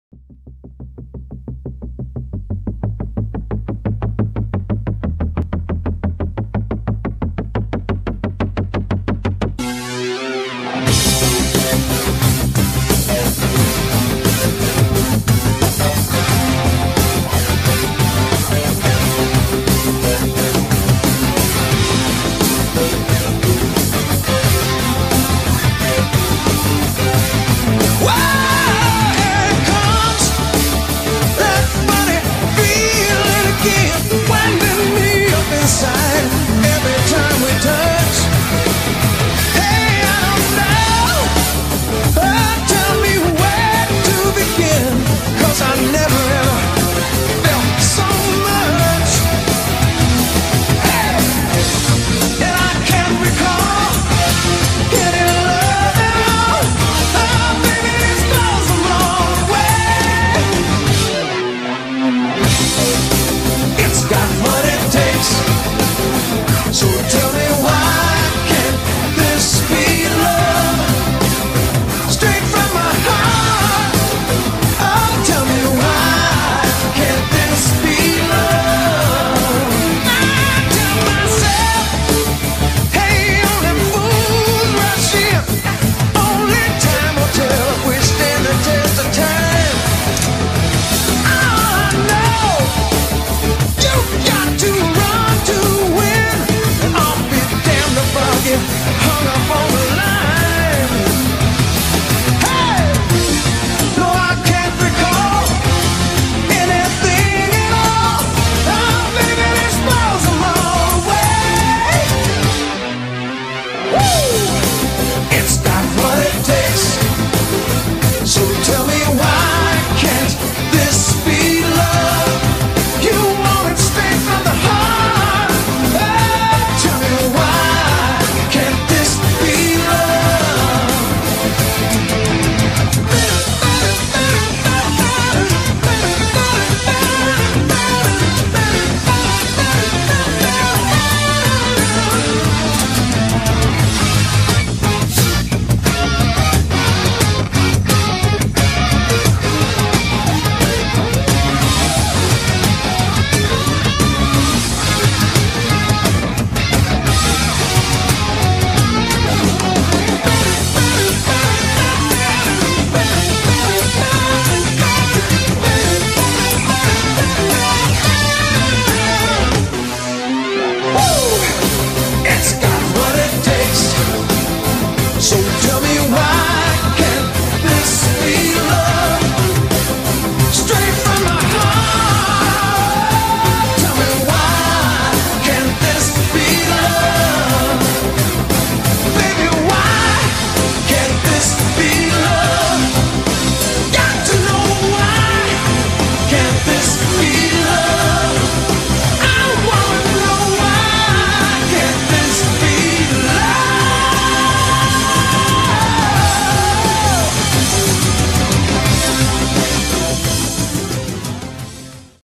Modding My Interface With EVH STRIPES!
fancied a change but there's no interface that i can find with evh stripes so I thought I would have a go at modifying my interface.
materials needed:
sandpaper fine
white spray
red spray
black spray
primer
clear coat optional I suppose
masking tape